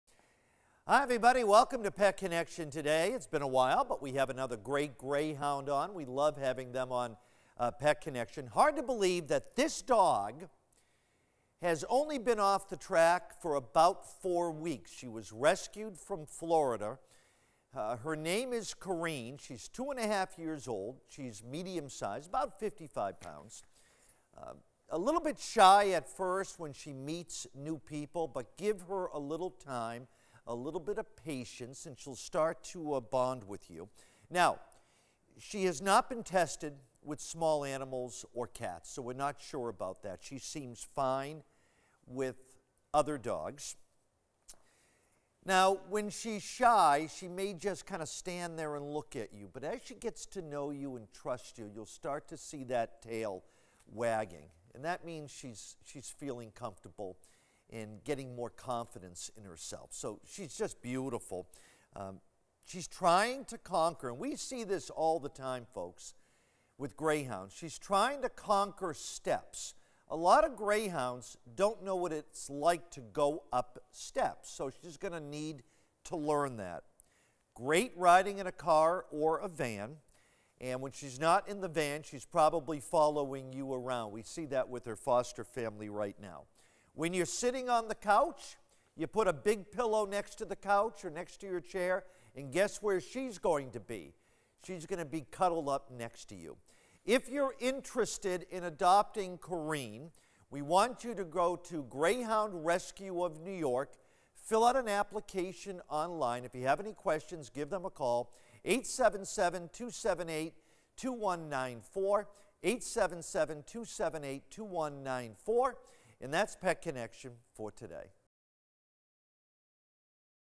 Pet Connection Corrine 11-19-20
Its about time !  Awesome Greyhound !
It will be like adopting a big puppy...still learning what love and a home is all about. Meet "Corrine". NO MORE running on a track for this girl.

Pet Connection 11-19-20

Corrine  is a tiny Red Fawn female  bundle of energy,   mixed with curiosity.
She was rescued off a Florida track 4 weerks agho and is adapting very well.

Corrine weighs 55 pounds, about 25 ½ inch height; is young at 2 ½ year old, needs to be tested with small animals/cats . .good so far with other dogs.

Corrine is shy in the beginning  and will stay close to you until she feels  less scared and her curiosity  kicks in and Corrine shows that with her tail  beginning to do a slow  wag

  Corrine  Is trying her best to conquer going up steps,   is  good at riding in the van  and when not following you everywhere

She enjoys a nice soft pillow by the side of your chair.

Greyhound Rescue of NY Inc. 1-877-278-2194

Fill out an application on line so you can set up a meet and greet.